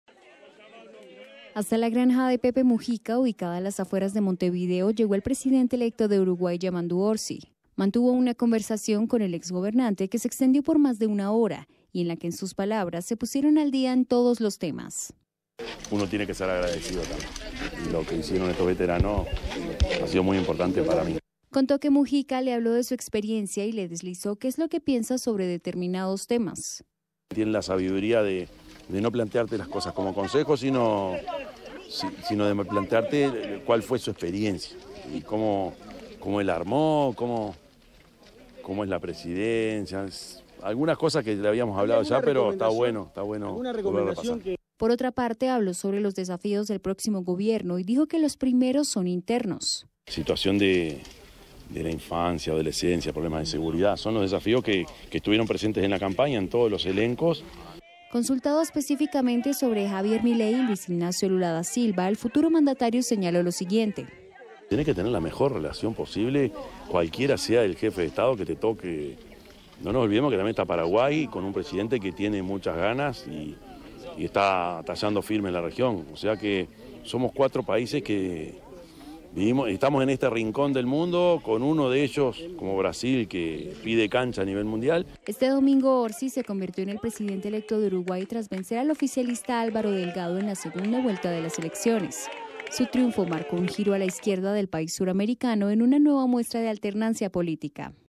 URUGUAY | Yamandú Orsi visitó a Pepe Mujica
👉 El presidente electo de Uruguay, Yamandú Orsi, visitó a su mentor, el exmandatario José Mujica, luego de la victoria electoral que permitirá el regreso del Frente Amplio al gobierno.